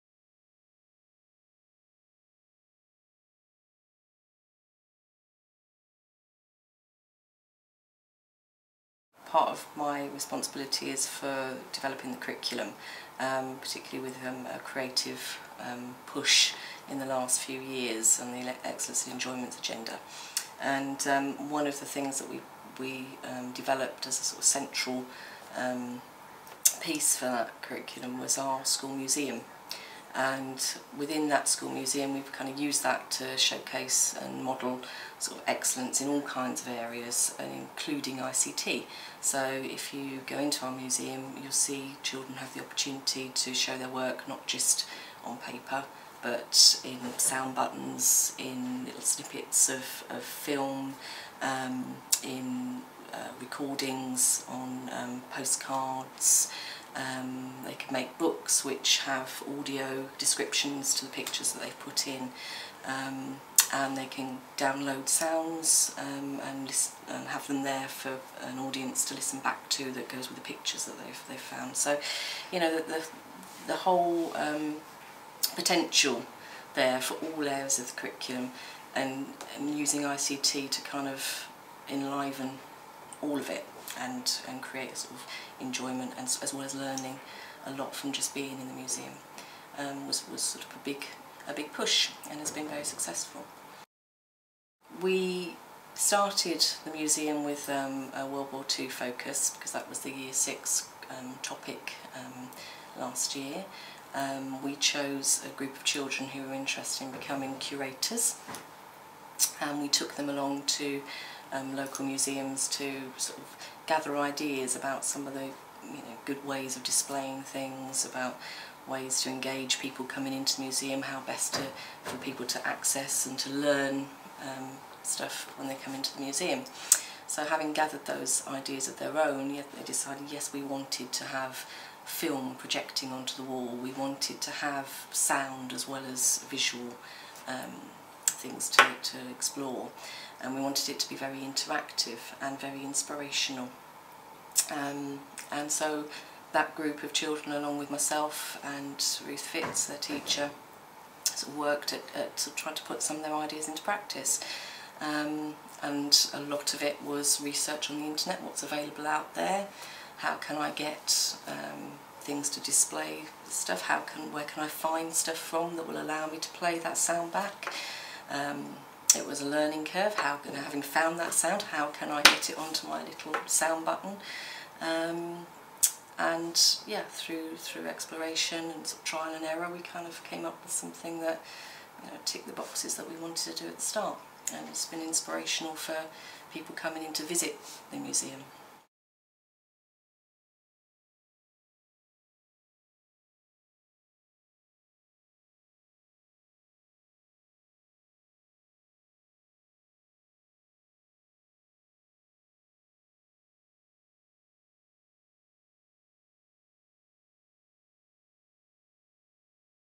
Creative Use of ICT: Museum at West Thornton primary Croydon
Watch this video to see an example of how West Thornton Primary School in Croydon has used ICT creativity to enhance their museum project.